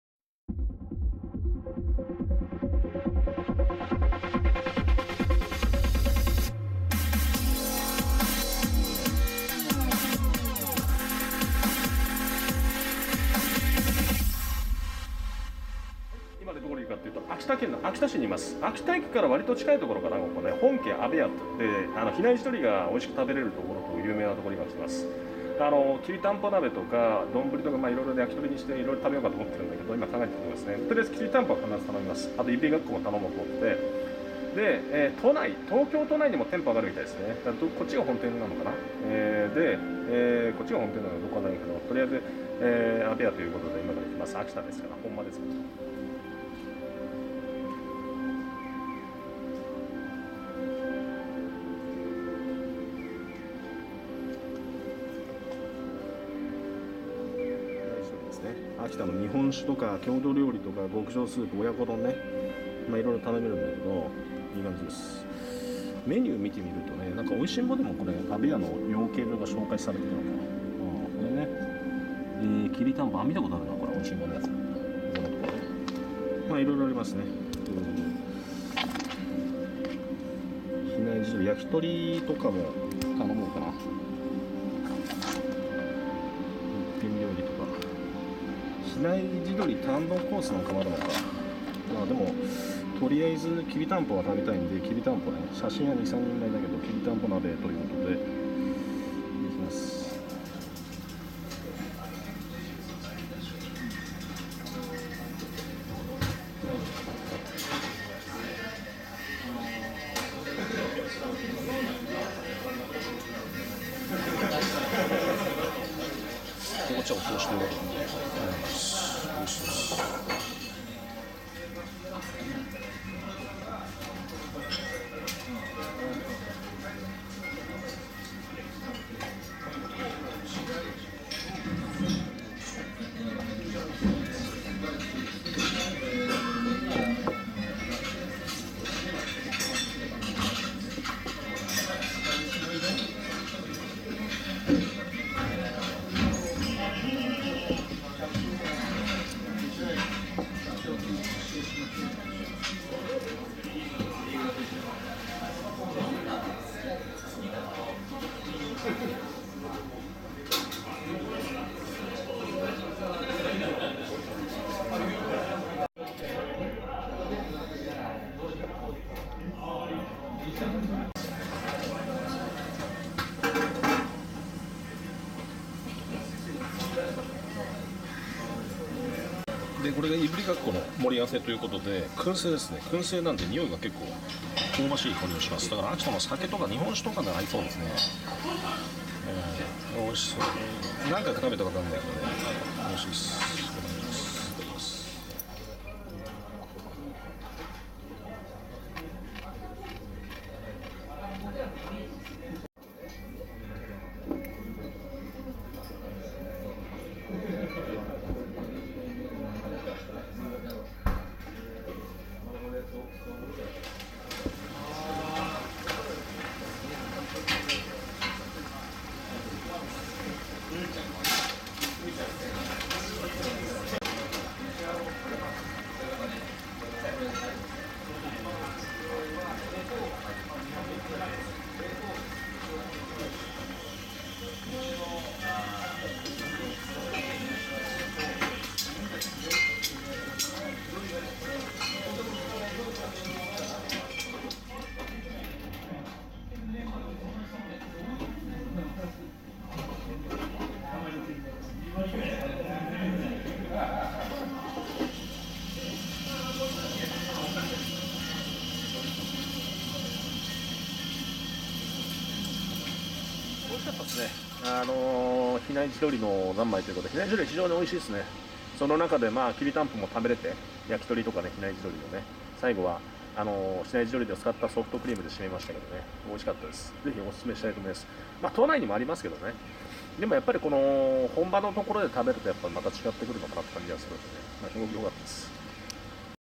【秋田料理】秋田市の「本家あべや」秋田名物の比内地鶏、きりたんぽ、いぶりがっこ堪能Gourmet of Akita prefecture at Honke-Abeya,Akita city,Japan
【秋田県秋田市の「本家あべや」秋田比内地鶏生産責任者の店 で秋田名物の比内地鶏堪能】
I ate the gourmet of Akita prefecture, using the special chicken called for Hinaidori. I ordered total 6 dishes and It was so delicious and the cost is about 50USD including drinks.
昨晩は秋田市内の比内地鶏の有名店、本家あべやにて比内地鶏料理を堪能しました。
比内鶏は、主に秋田県北部・米代川流域(比内地方)で古くから飼育されている家禽。天然記念物に指定されています。
薩摩地鶏、名古屋コーチンと共に、日本三大地鶏と言われており、歯ごたえがあり、濃厚。非常に美味です。
昨晩の献立は下記
・比内地鶏のモツ煮
・いぶりがっこ
・比内地鶏の焼き鳥
・比内地鶏の卵黄
・比内地鶏のパテ
・比内地鶏のきりたんぽ鍋
・比内地鶏の卵ソフトクリーム
飲み物合わせての費用は合計で5600円程。
ちなみに、本家あべやは都内にも複数店舗がありますので
首都圏在住者はご利用ください。